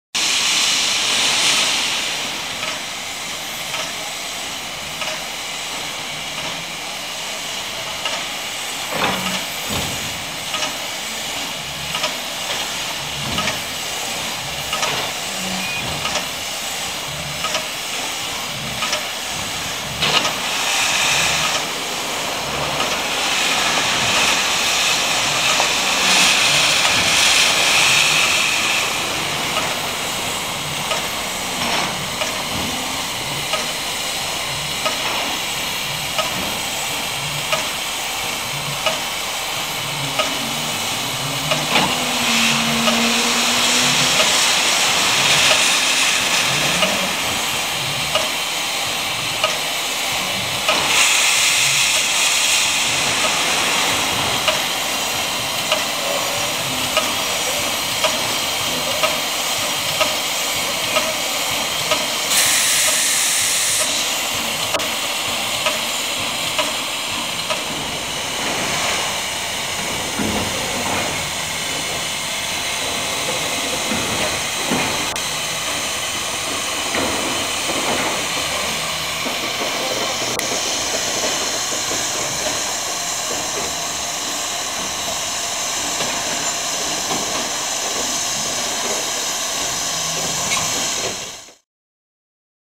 534.0432 | výjezd na výkon
534.0432 vyjíždí z rotundy na vozy a pak na filmovací místo pro štáb filmu CHILD 44